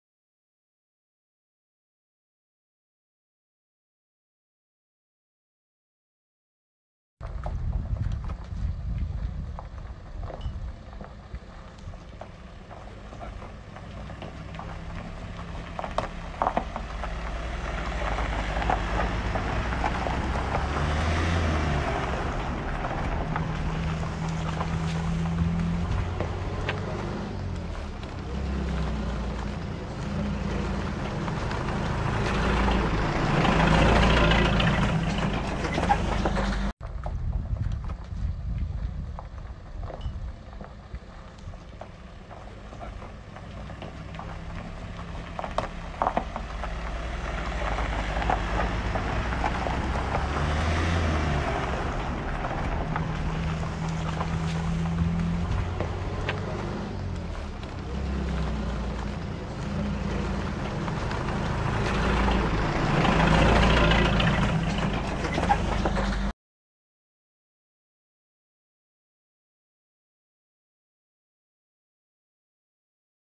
ShkodraWeb I Transportohen nga Vrithi në Shkodër trupat e pajetë të Klement Pepaj dhe Vilson Çutaj
Detaje të tjera dalin në dritë pas vrasjes së dyfishtë në fshatin Vrith të Malësisë së Madhe. Ka qenë babai i Danjel Pepajt, Rrok Pepaj ai që ka dërguar policinë pranë parcelave me kanabis.